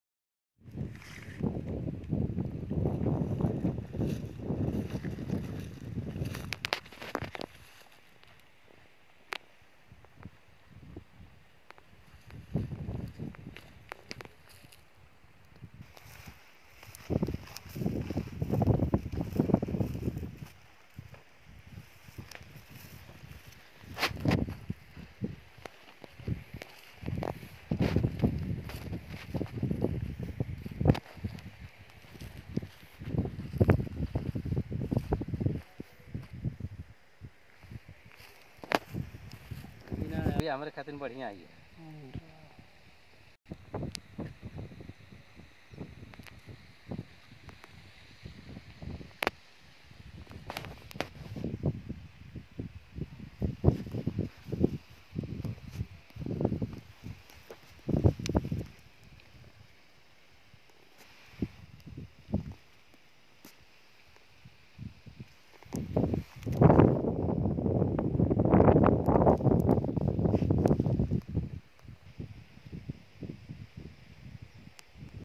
INDRADHNUSH PROM
इंद्रधनुष प्रोम खाद DAP का विकल्प
प्रोम खाद का सभी फसलों मे बुआई के समय या सिचाई से पहले प्रयोग किया जा सकता है
50 से 100kg एकड़ या आवश्कता अनुसार प्रयोग करें
गेहूँ, धान, सरसो, आलू, मटत, केला, एवं पेड़ पौधे मे भी